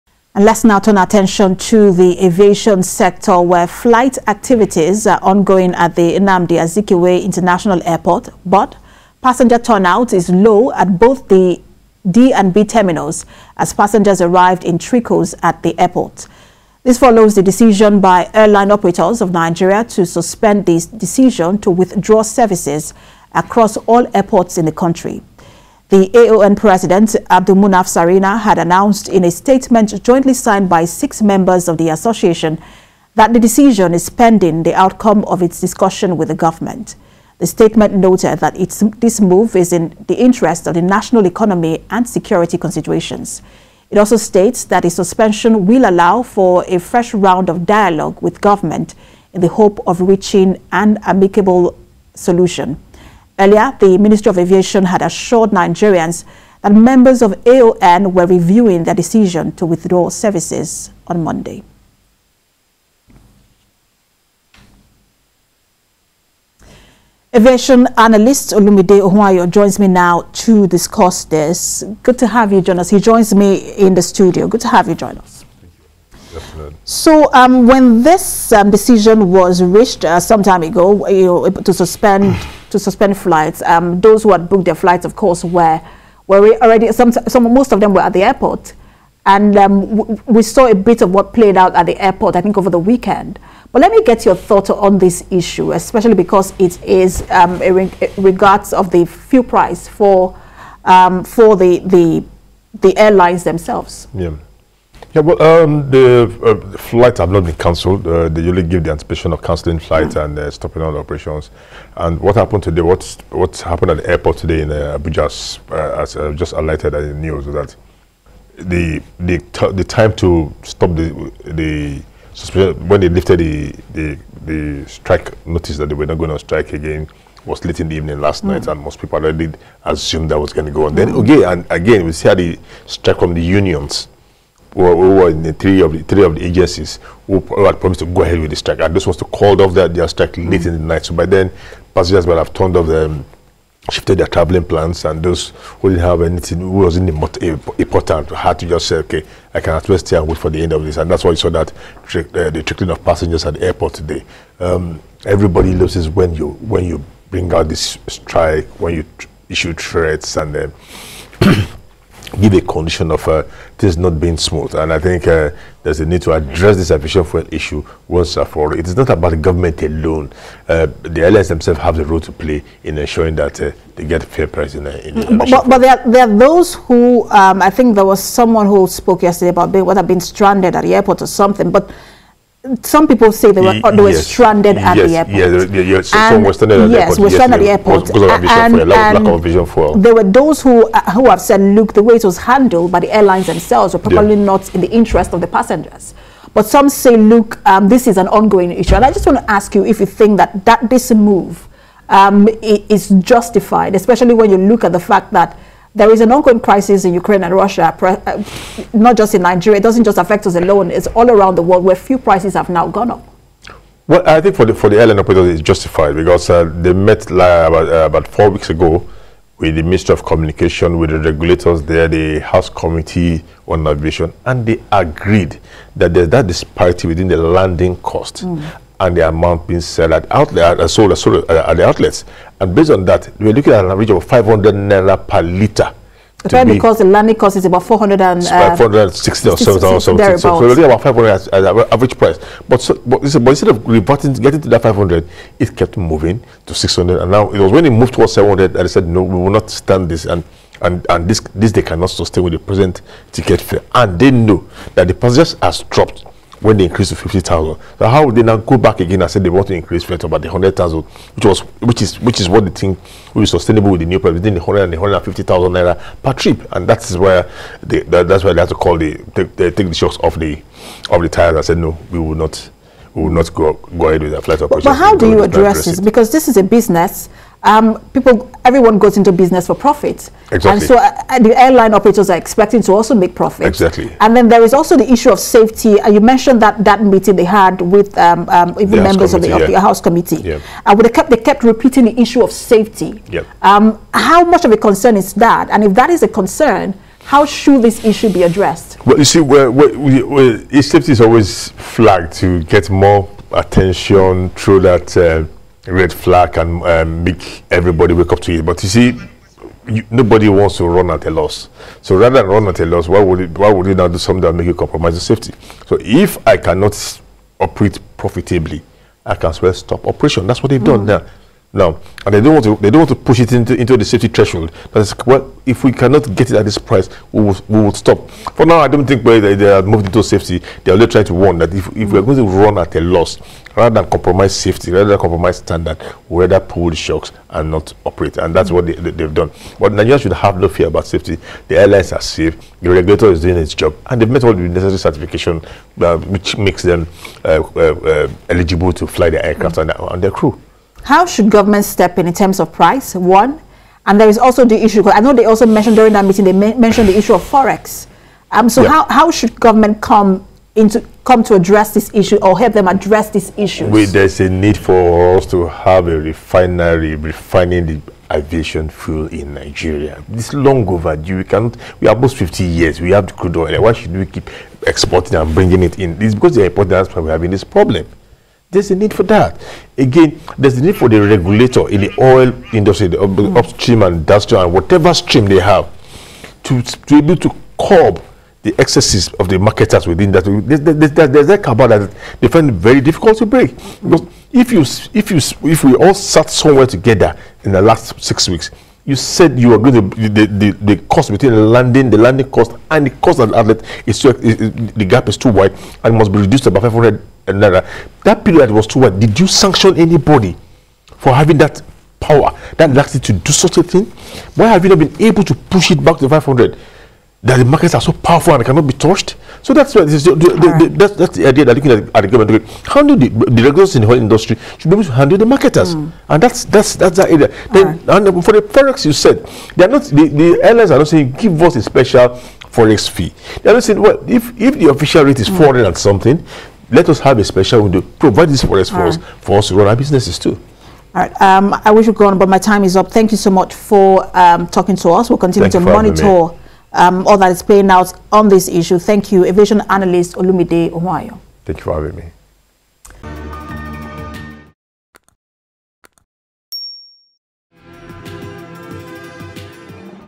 Association Of Airline Operator Suspends Strike
Flight activities are ongoing at the Nnamdi Azikiwe International airport but passenger turnout is low at both the D and B terminals as Passengers arrived in trickles at the airport.
This follows the decision by Airline Operators of Nigeria to suspend the decision to withdraw services across all airports in the country.
AON President, Abdulmunaf  Sarina, had announced in a statement jointly signed by six members of the association that the decision is pending the outcome of its discussion with the government